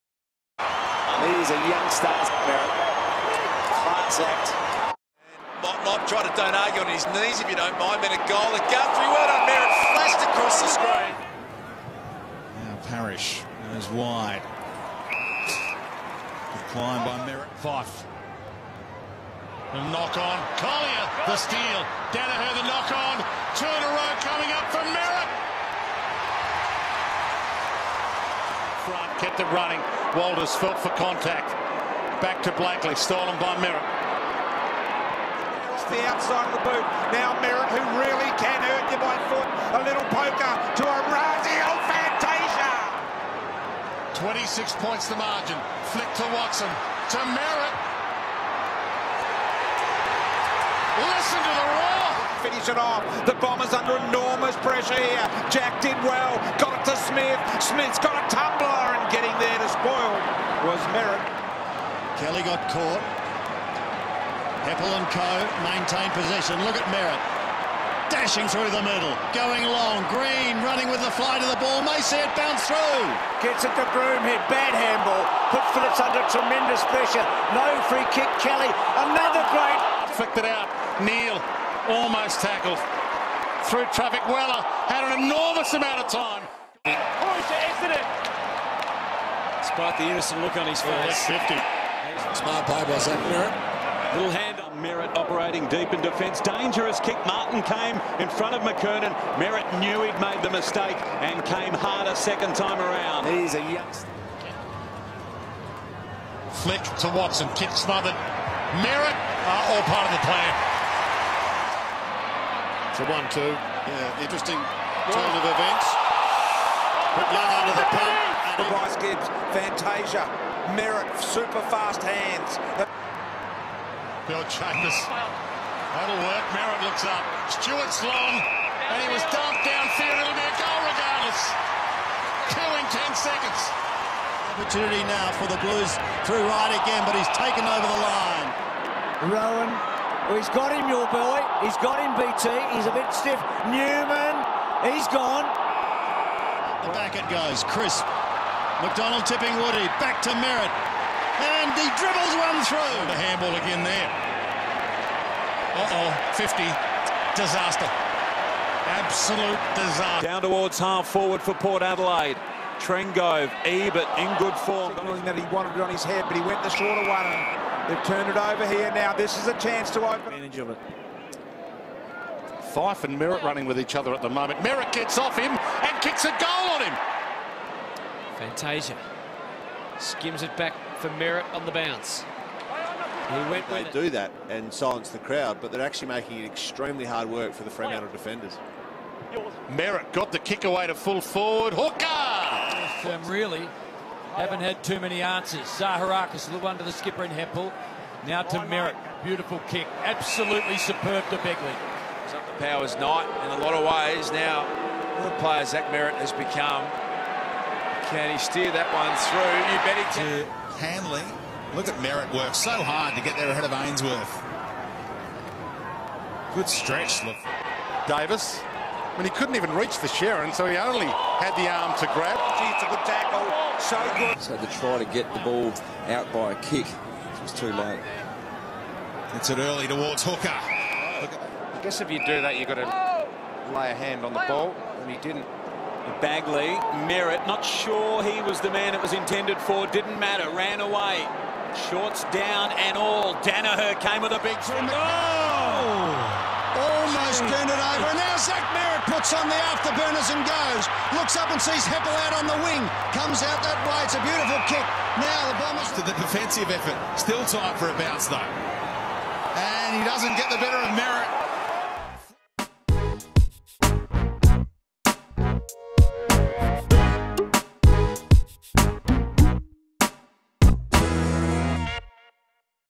Zach Merrett Highlights of 2017
Zach Merrett has been named the vice captain of the 22under22 squad and has been named in the All Australian squad for 2017. He has also been chosen to represent Australia in the International Rules Series and last year this youngster won Essendon's best and fairest. Zach's commitment to developing his game is what makes him an incredible player and I'm sure that in the near future we will see him win a Brownlow.